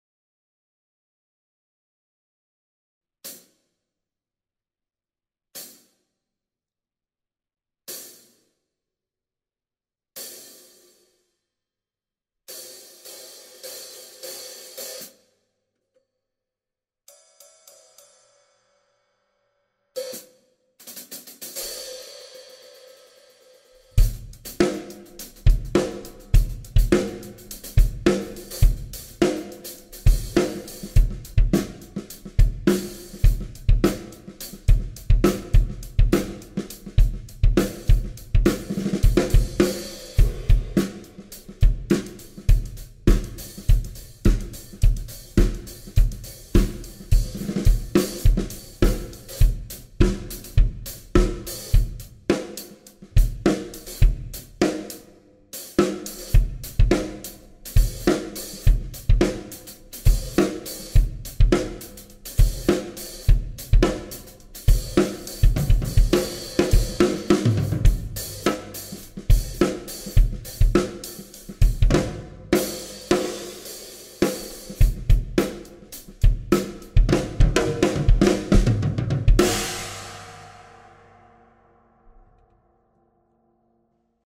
Red Cymbals 15" Phil's 'Phifteens' Flat Bottom Hi-Hats - Collab with Phillip Schinbein
This is a demo of the 15" Phil's 'Phifteens' Flat Bottom Hi-Hats the weigh approximately 1150 grams (top) and 1600 grams (bottom).

These hi hats have a heavier but flat bottom to our standard hi hats. They are a part of the 'Phil's Phlats' Collab with Phillip Schinbein to make a series of flat bell cymbals such as the 23" Phlat Bell China and Phlat Bell Ride.